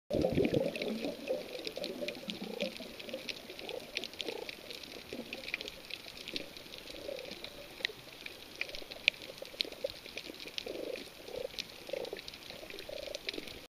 Sea Cucumber Purring
sounds like a cat's purr.